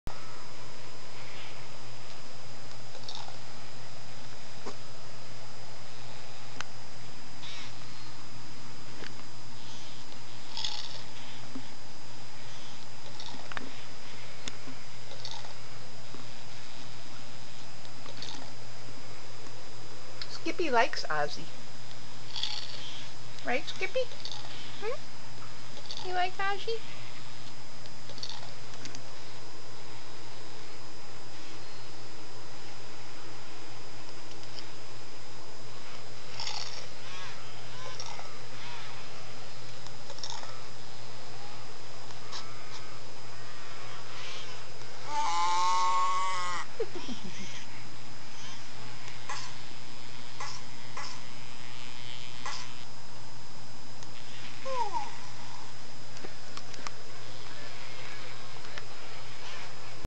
Skippy the Kitty sits next to Ozzy - listen to crackling sound at end (Ozzy's neck?) Aug 17 2011
Ozzy eating Skippy smelling bandana, sniffing Ozzy, sitting next to him on bandana, petting Oz, cries, then sighs..

Cracking sounds towards end of clip  (neck?)